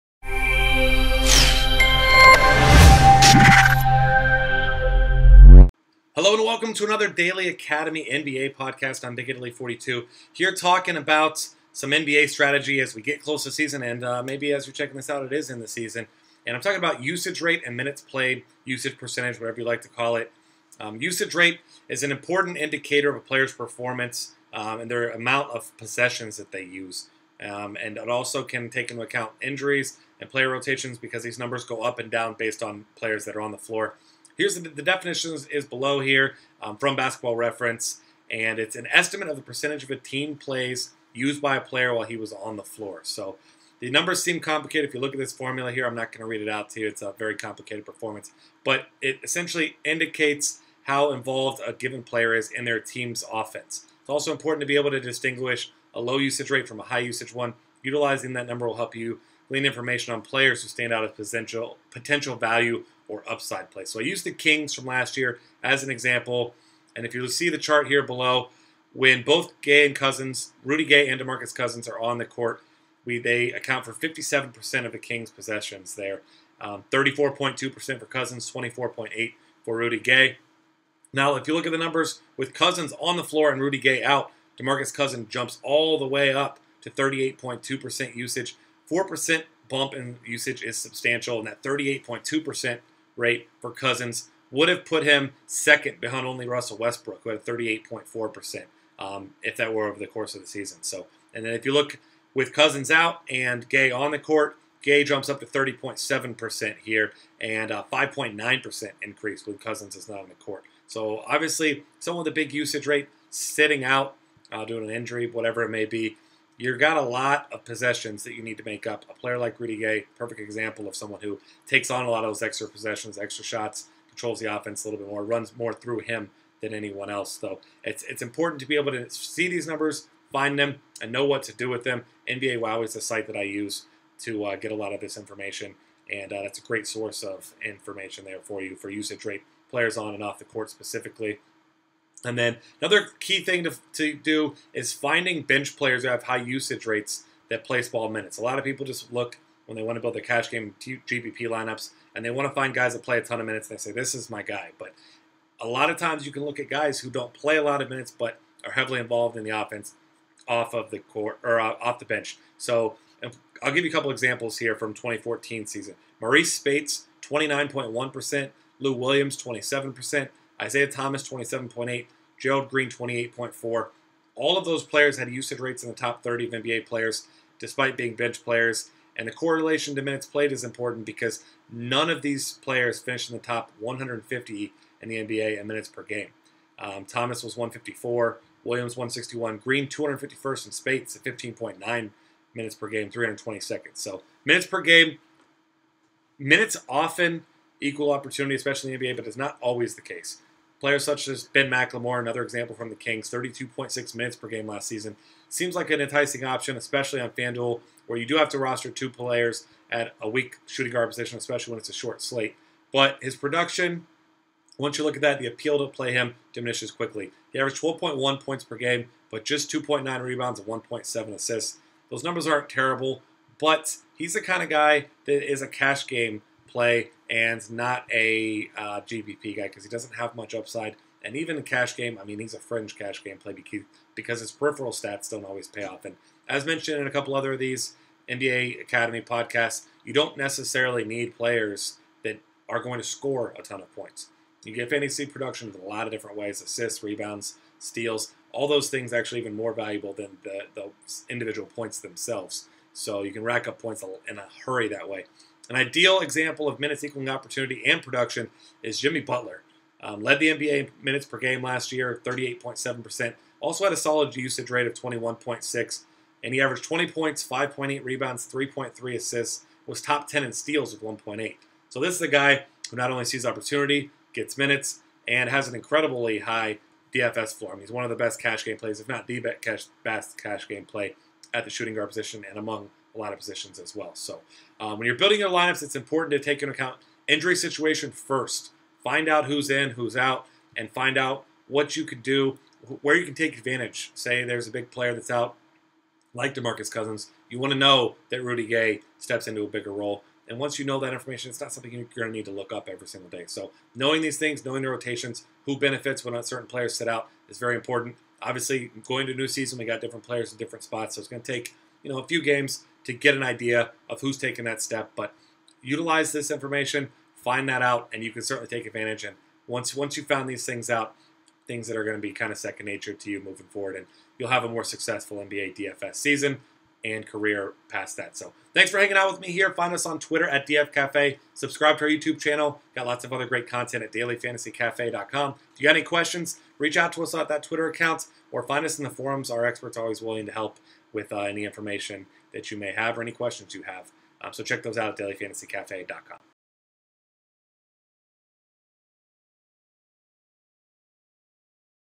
Usage Rate and Minutes in NBA DFS
- Our expert @BigItaly42 breaks down how to utilize Usage Rate and Minutes played to help you gain an edge in your NBA DFS contests! Follow Cafe experts on Twitter @dfcafe!